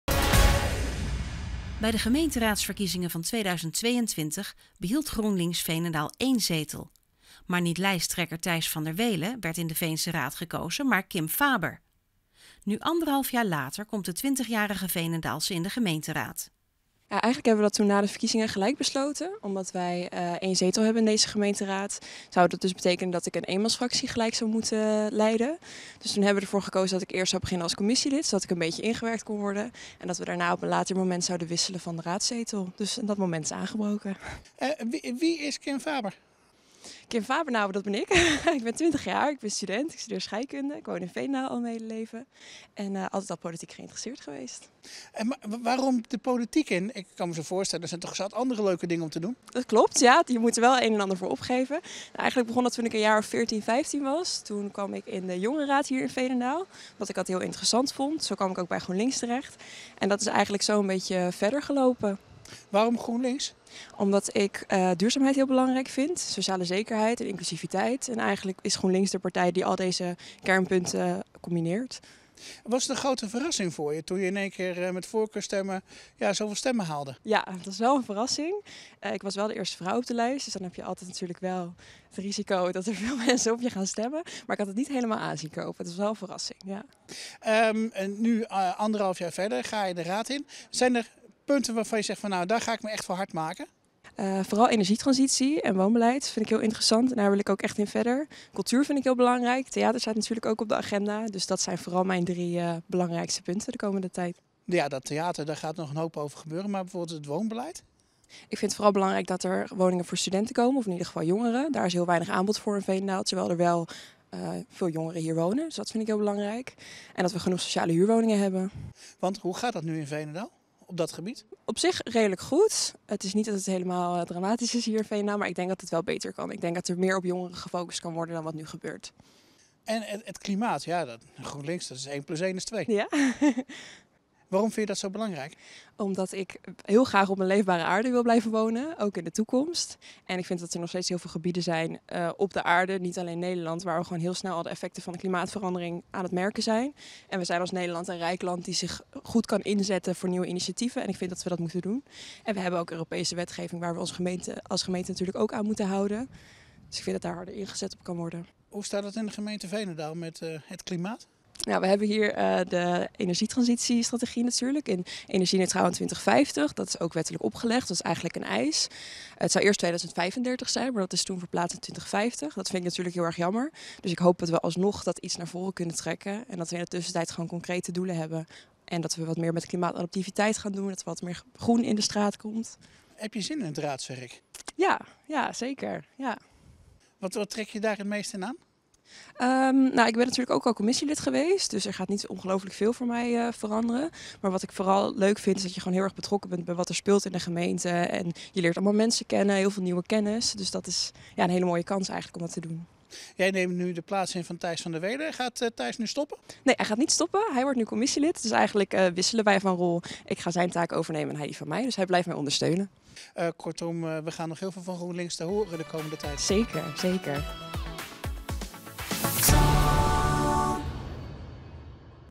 Kim Faber nieuw raadslid GroenLinks Veenendaal
Bij de gemeenteraadsverkiezingen van 2022 behield Groenlinks Veenendaal 1 zetel, maar niet lijsttrekker Tijs van der Weele werd in de Veense raad gekozen maar Kim Faber. Nu anderhalf jaar later komt de 20-jarige Veenendaalse in de gemeenteraad.